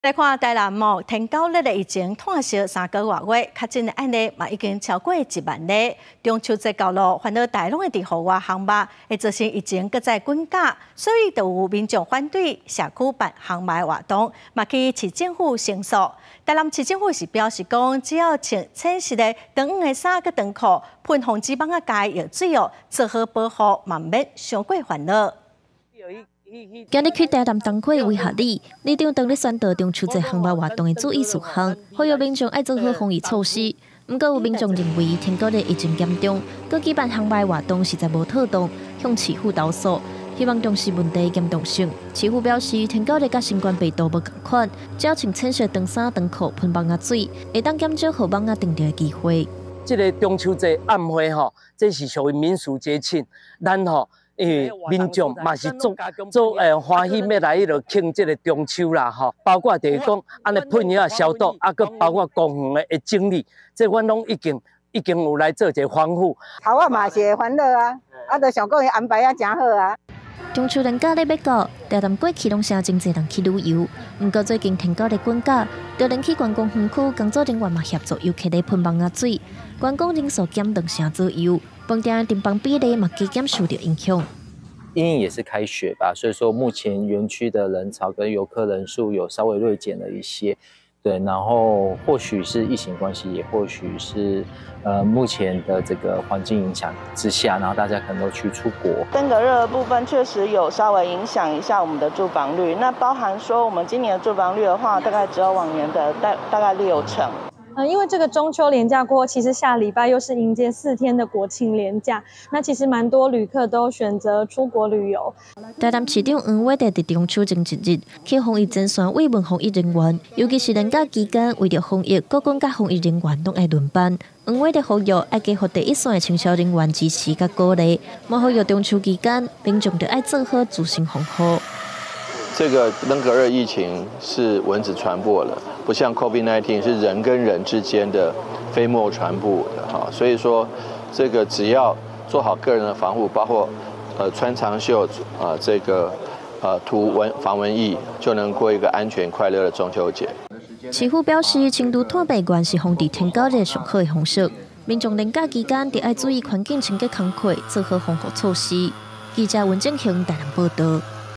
臺南超過萬人確診天狗熱 驚中秋害疫情閣再湠 | 公視台語台 | 20230928
台南天狗熱的疫情炭燒3個多月，確診的案例已經超過一萬例，中秋節要到囉，煩惱大家在戶外燒肉，會造成疫情閣再滾絞，所以有民眾反對社區辦烤肉活動，也去市政府申訴，台南市政府表示，只要穿淺色長衣、長褲，噴防止蚊子咬的藥水，做好保護也不免太煩惱。

四節新聞 ：　逐工（每天）09:00 / 12:00 / 15:00（拜一至拜五） / 19:00